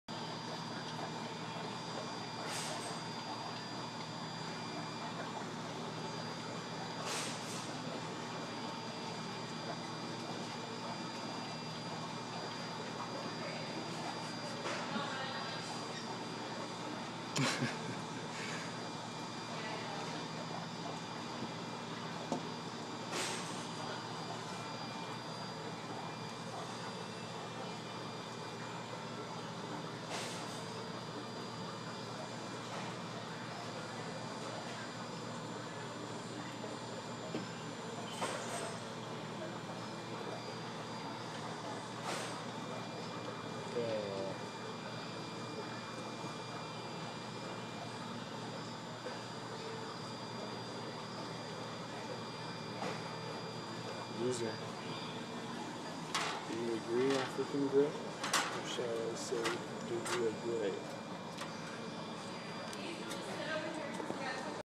Ferret Loves Him His Corn
Ferret at my work station got a new toy. A cameo by Becks, the baby African Grey.